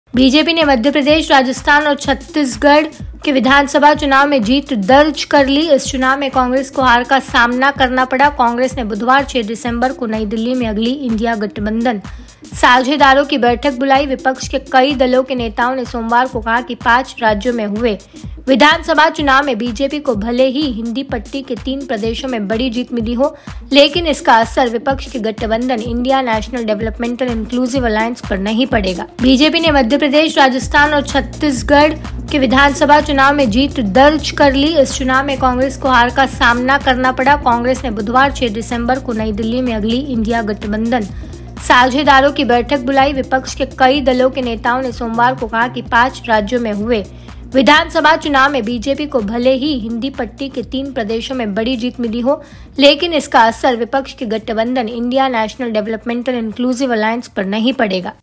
BJP की जीत के बाद कल इंडिया गठबंधन की बैठक होने वाली है
बीजेपी ने मध्य प्रदेश, राजस्थान और छत्तीसगढ़ के विधानसभा चुनाव में जीत दर्ज कर ली है. इस चुनाव में कांग्रेस को हार का सामना करना पड़ा है. कांग्रेस ने बुधवार, 6 दिसंबर को नई दिल्ली में अगली 'इंडिया' (I.N.D.I.A) गठबंधन साझेदारों की बैठक बुलाई है. विपक्ष के कई दलों के नेताओं ने सोमवार को कहा कि पांच राज्यों में हुए विधानसभा चुनावों में BJP को भले ही हिंदी पट्टी के तीन प्रदेशों में बड़ी जीत मिली हो, लेकिन इसका असर विपक्ष के गठबंधन ‘इंडियन नेशनल डेवलपमेंटल इन्क्लूसिव अलायंस’ (इंडिया) पर नहीं पड़ेगा.